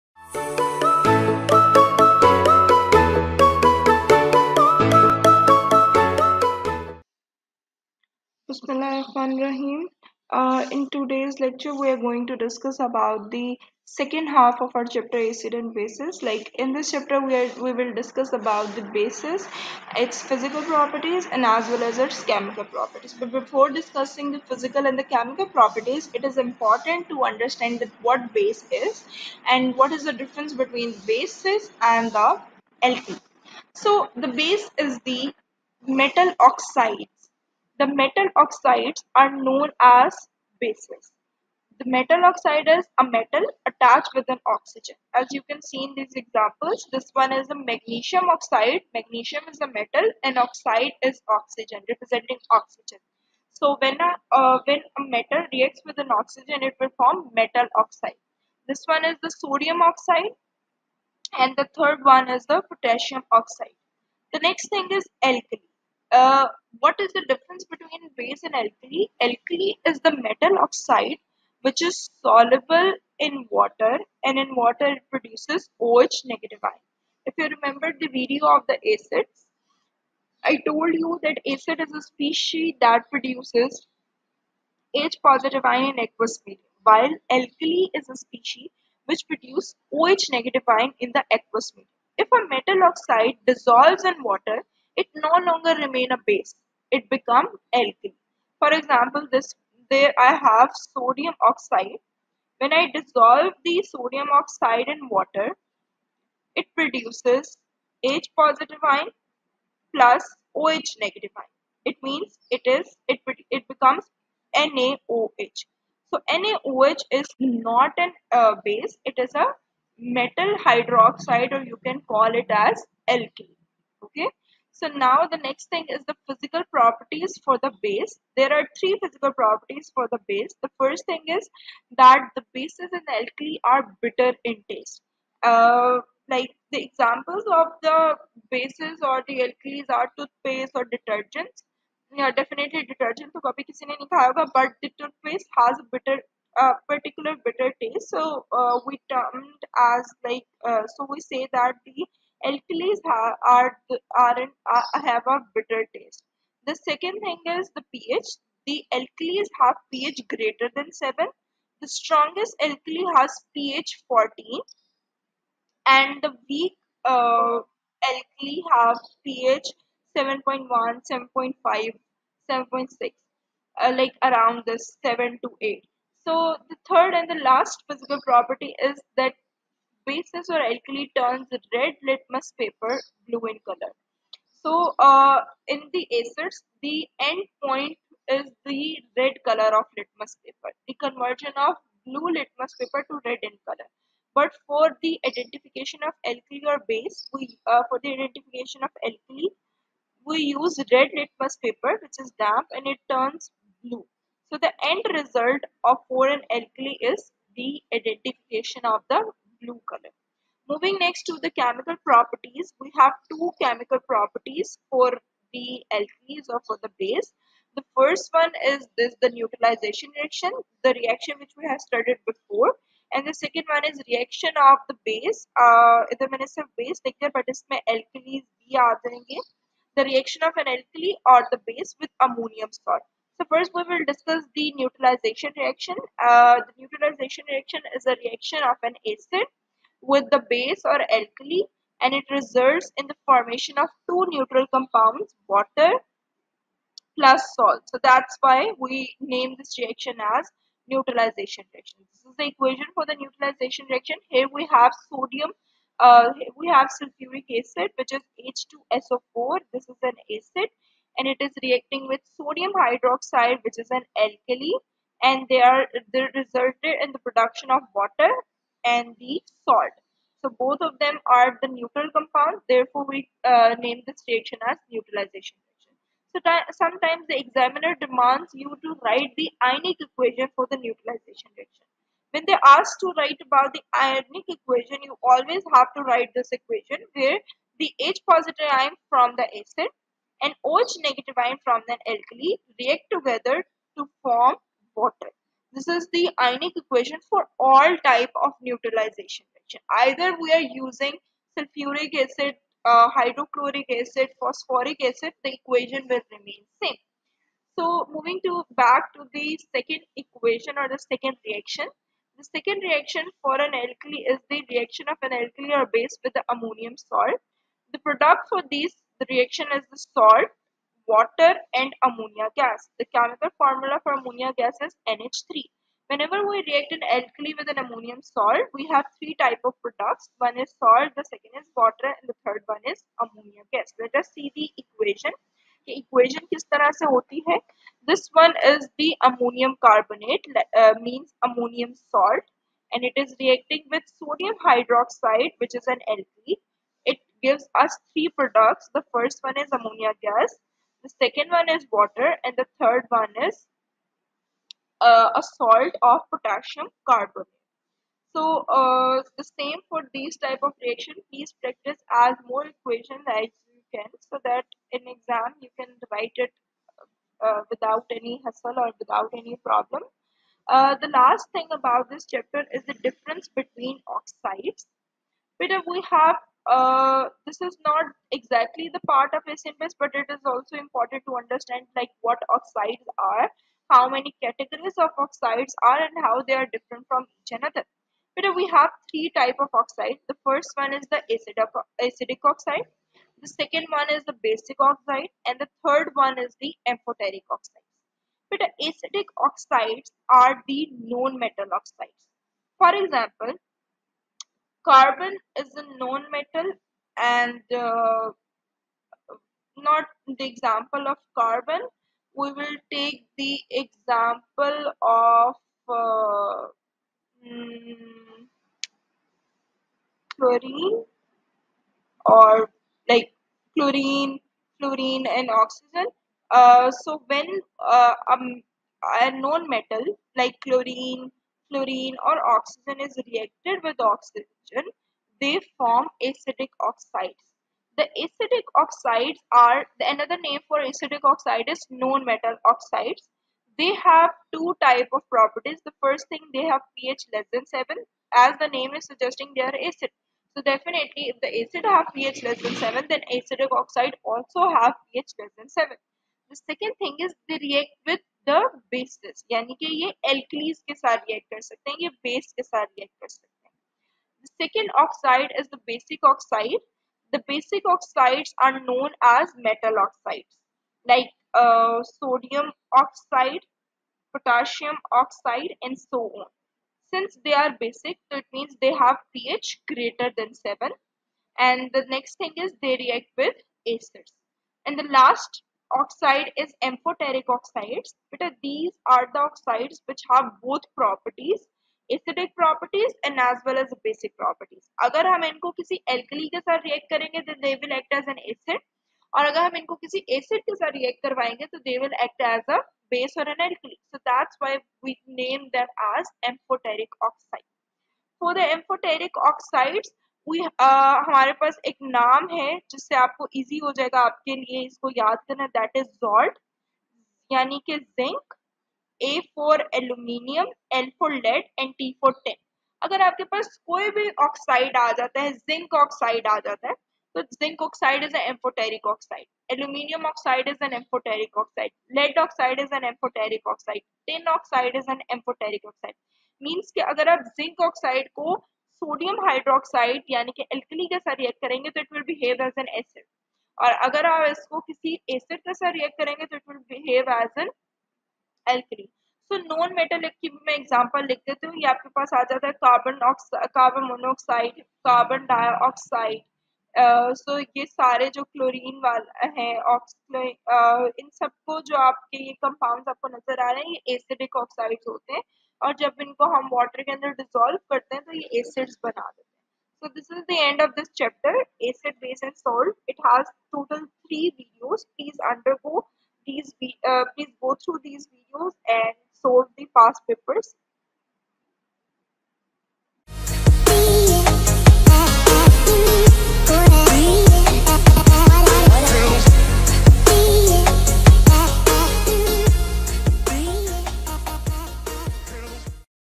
Chemistry -Acid and Base | Part 3 Alkali and oxides | O Level 5070 | Matric | IGCSE | Class 9th-10th
Acid, Base, Alkali and oxides concept made easy!
Watch this video till end to get the concept covered.

Use the comment section for questions!